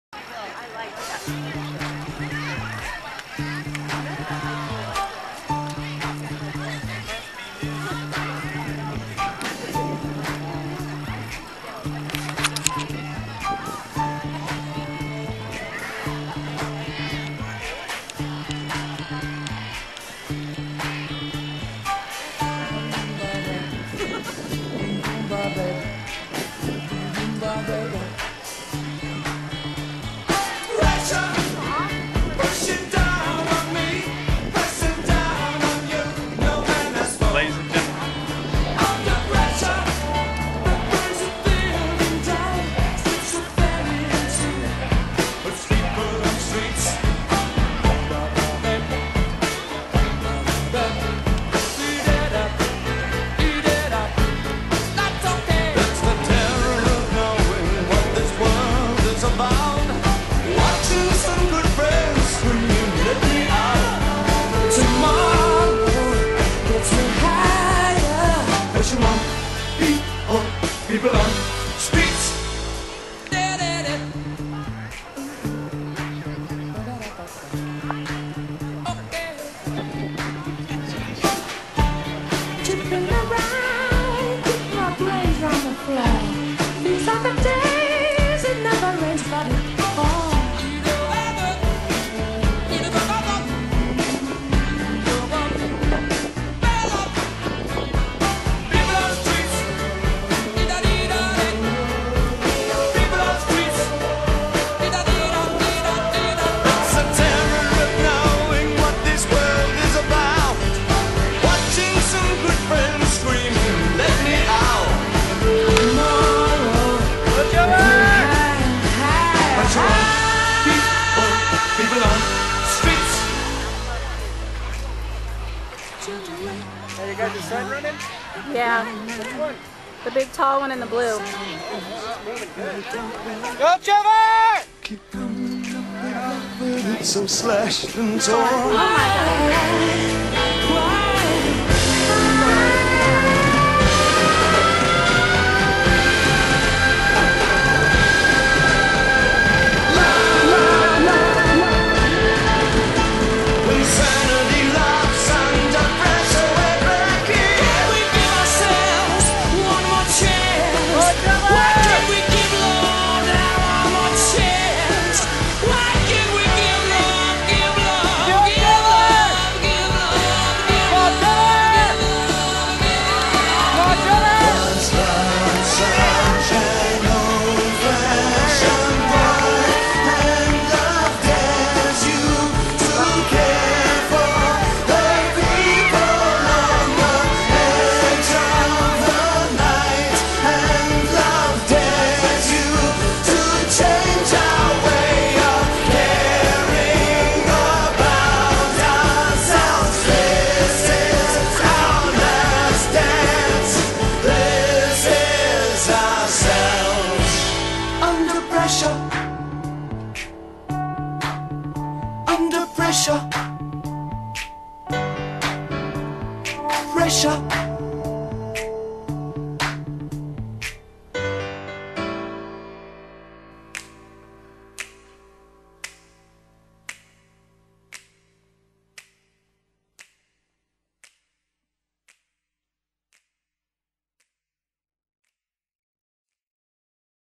The Track Meet Episode 1
Me running the 400 meters in a track meet